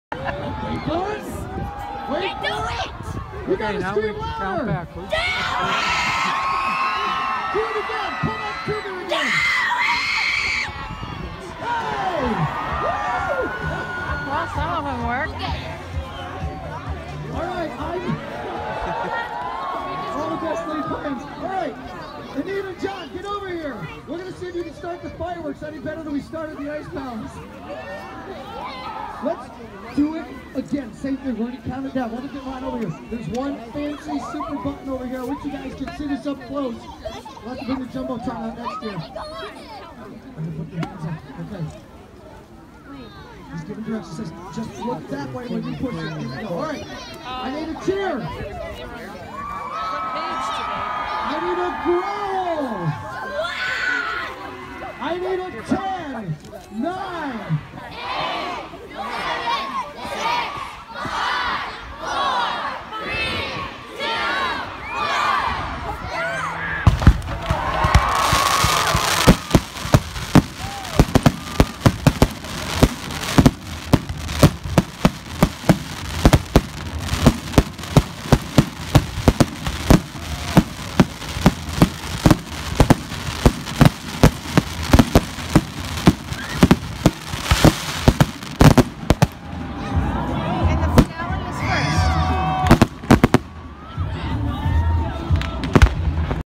Saranac Lake Winter Carnival 2017 Ice Palace Lighting
Every year Saranac Lake puts on a Winter Carnival.  This year's theme is Adirondack Wildlife.  This is the ice palace lighting.  For two weeks you can tour the ice palace.  The ice blocks are harvested from nearby Lake Flower and are approximately 10 inches deep.